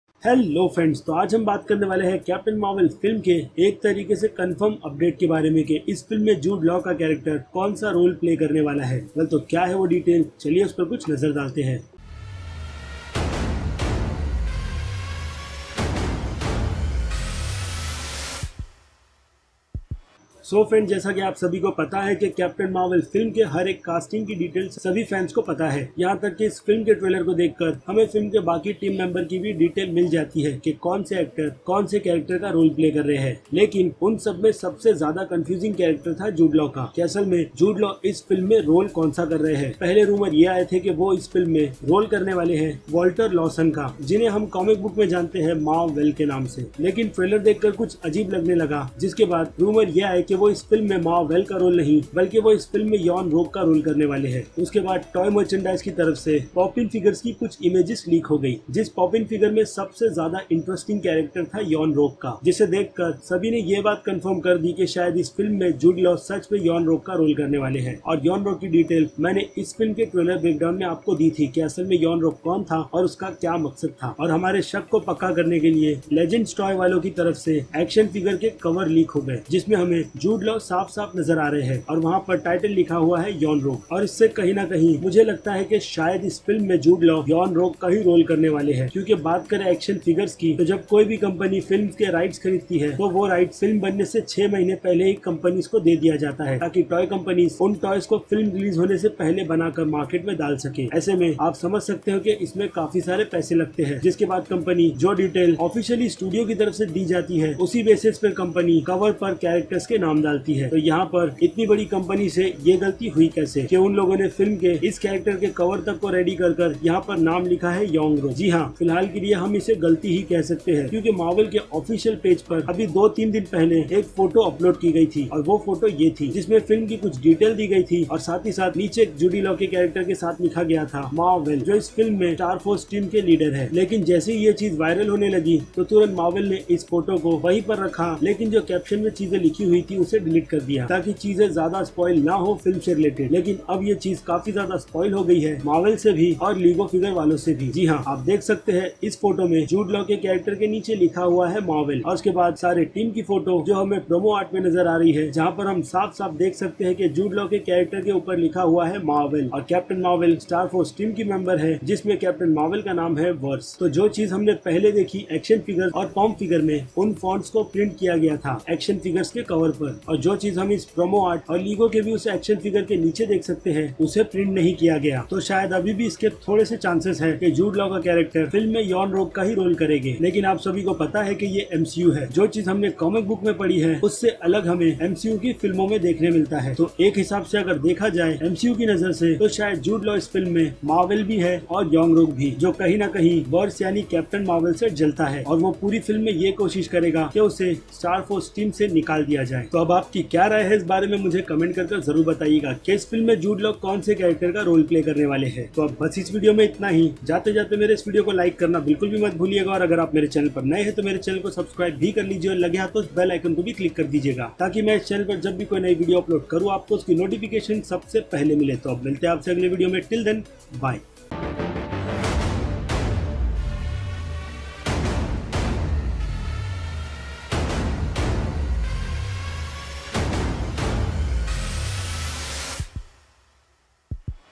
Captain Marvel Jude Laws Character Details LEAKED [Explained In Hindi]
Hello friends aaj hum baat karne wale hai captain marvel film ke Jude Laws ke Character ke bare me to kya hai woh chaliye dekhte hai